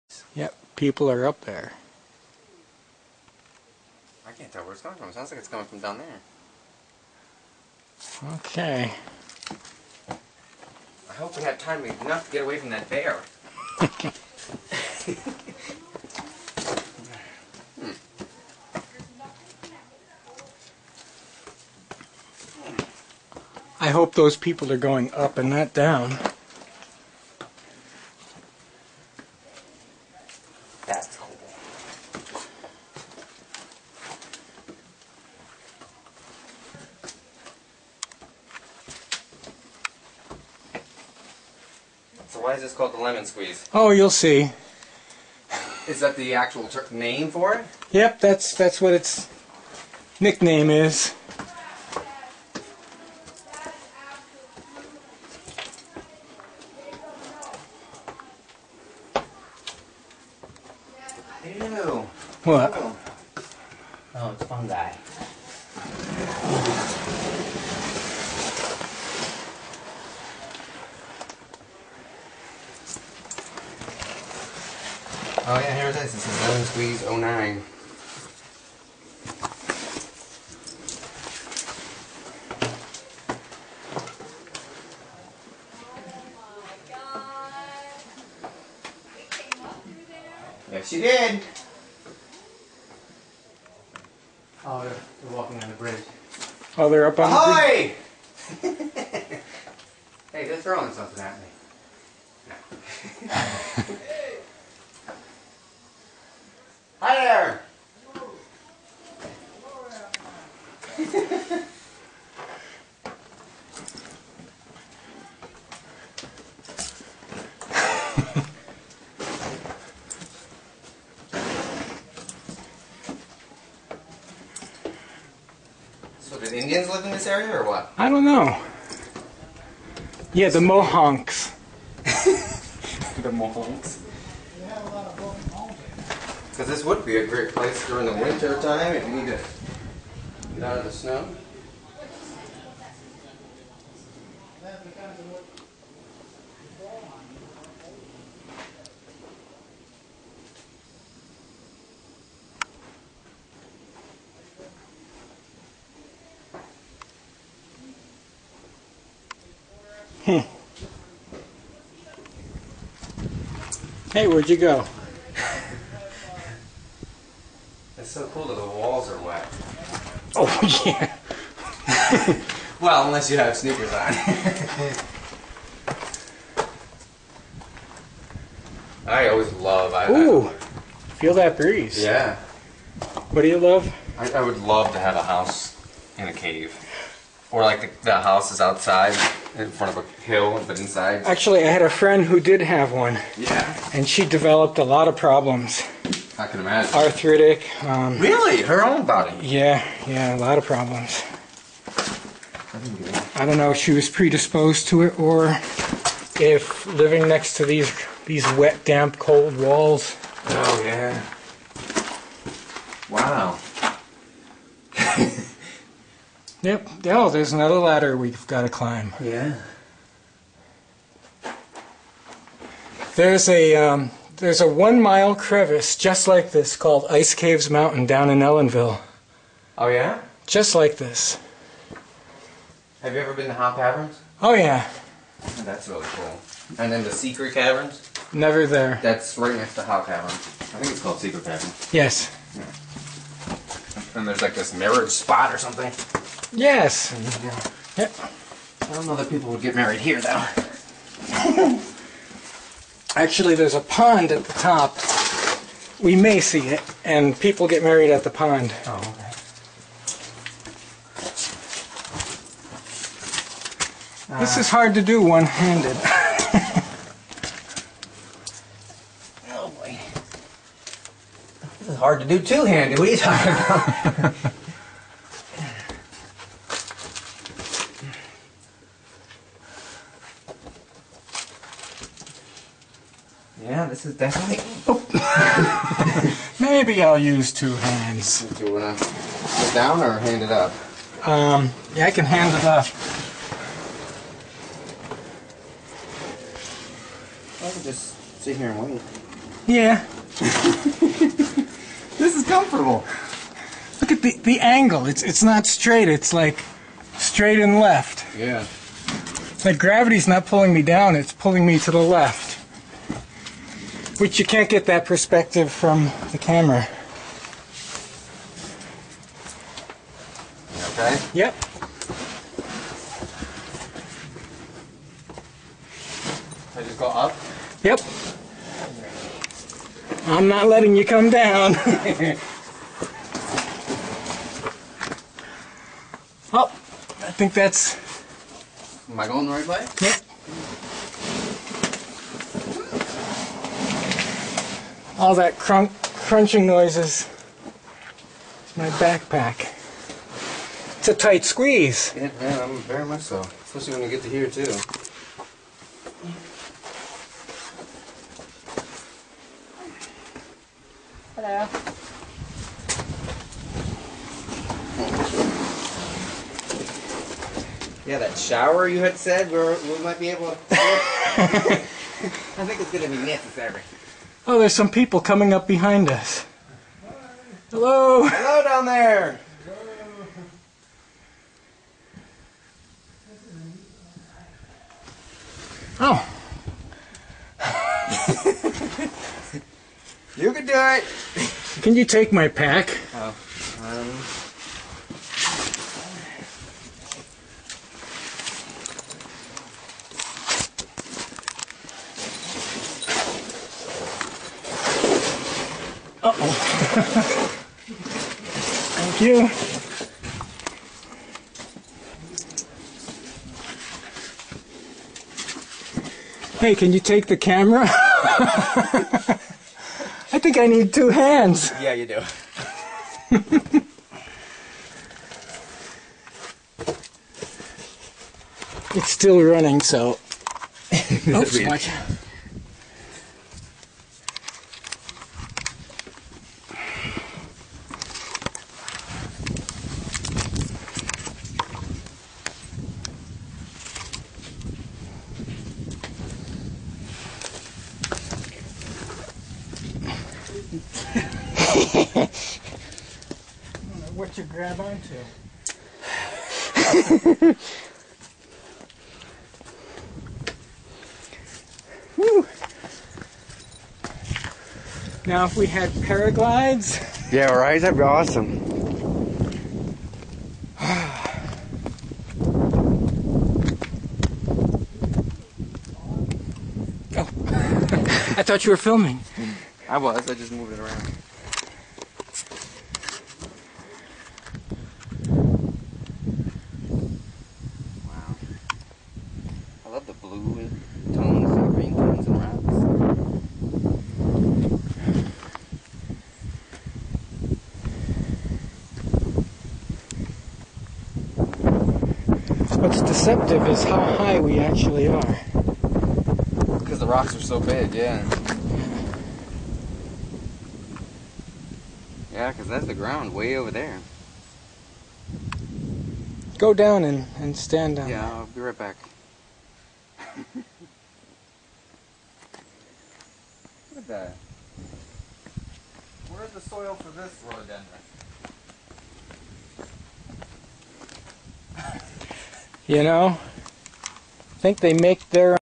lemon squeeze
Another idiot carrying a camera through the Lemon Squeeze at the Mohonk Preserve in New Paltz, NY. Malcolm Kogut